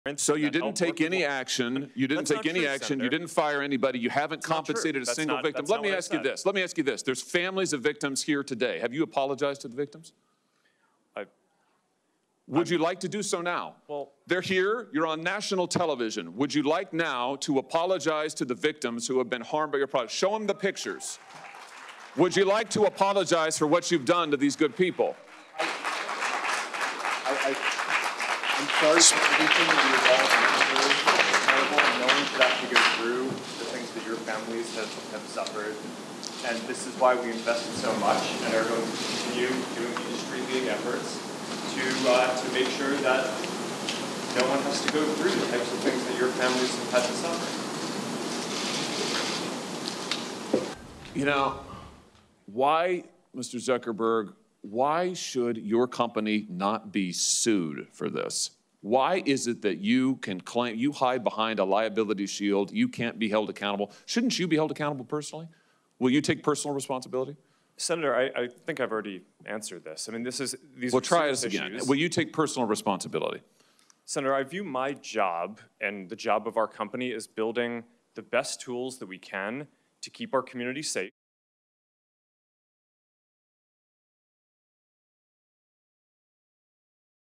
Mark Zuckerberg apologises directly to families of online harm victims in Senate hearing
Mark Zuckerberg, the chief executive of Meta, spoke directly to victims and their family members as he testified during the US Senate judiciary committee hearing on Wednesday.

After an intense line of questioning by the Republican senator Josh Hawley, who asked Zuckerberg if he would like to apologise to families of victims who were sitting in the audience holding photos of children they say died or were harmed due to his platform, Zuckerberg stood up and faced them.